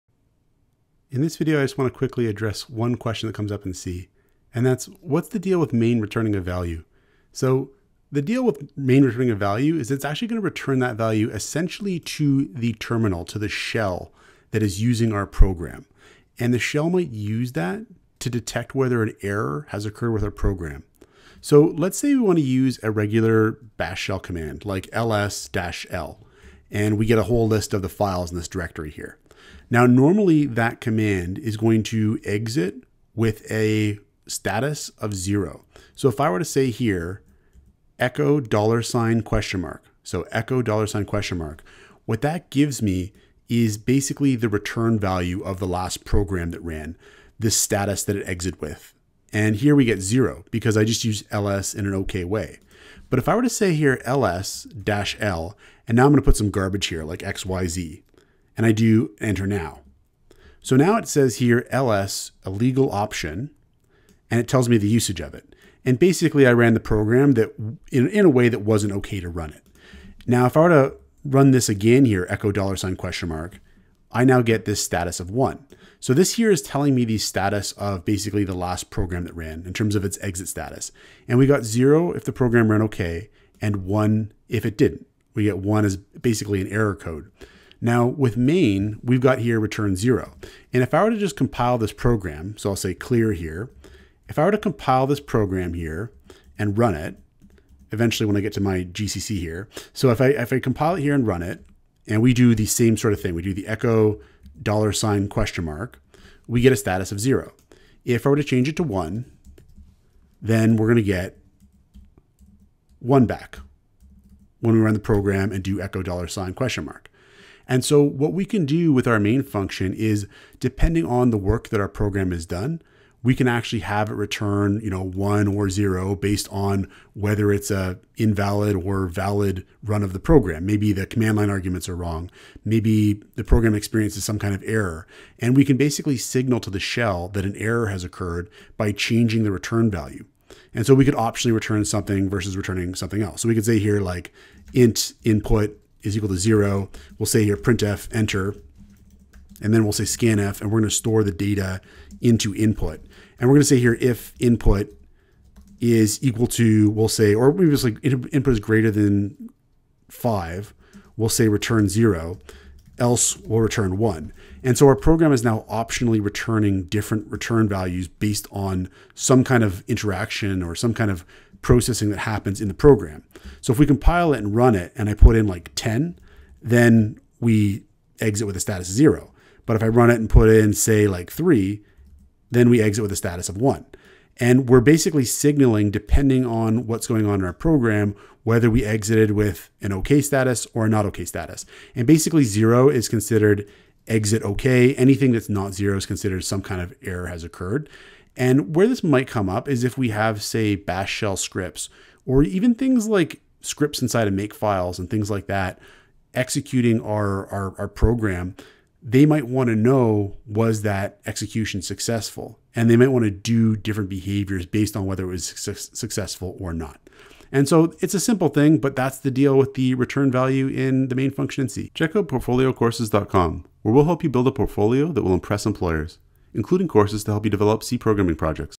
Main Function Return Value | C Programming Tutorial
An overview of main function return values in C. Source code to build a portfolio that will impress employers!